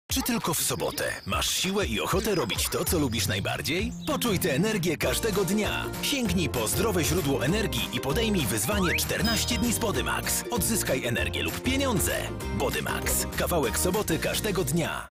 Bodymax. Kawałek soboty każdego dnia! - Zabawa z dziadkiem
Czy tylko w sobotę masz siłę i energię, by robić to, co lubisz najbardziej? Poczuj energię każdego dnia! Sięgnij po zdrowie źródło energii i podejmij wyzwanie 14 dni z Bodymax!